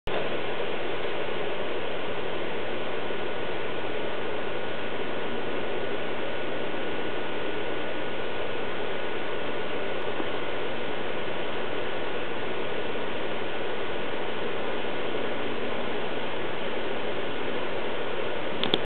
Inch Worm
This inch worm was crawling around on my porch so I made this video.